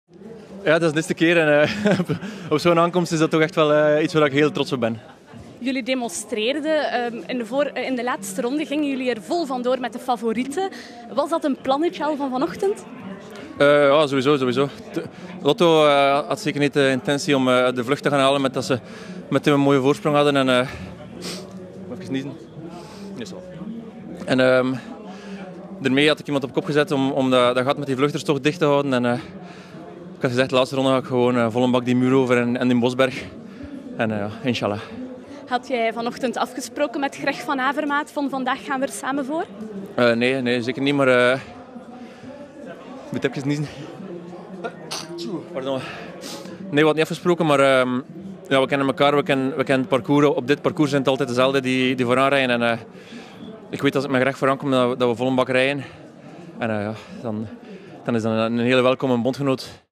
Ritwinnaar Oliver Naesen: "Ik ben erg trots"
Eerste reactie van Oliver Naesen na zijn overwinning in de slotetappe van de BinckBank Tour 2019.